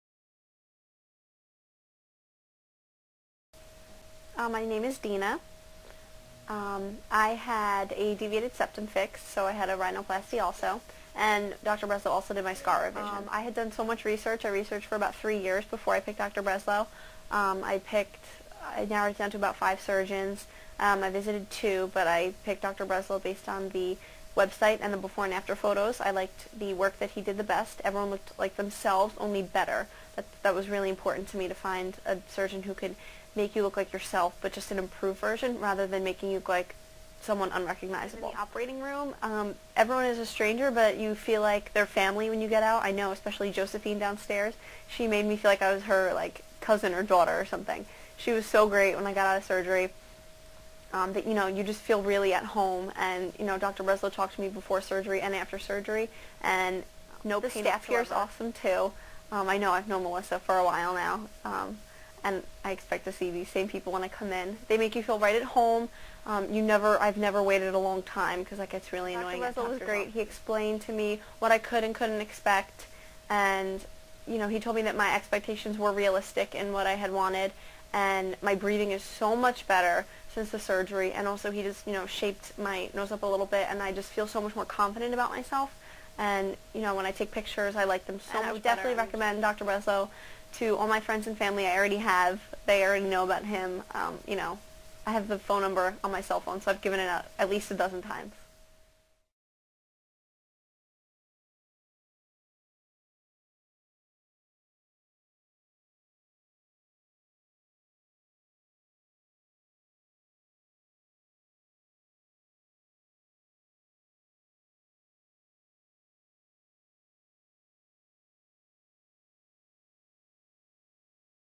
Deviated Septum & Rhinoplasty
Dina discusses her experience at The Breslow Center regarding her Rhinoplasty & deviated septum.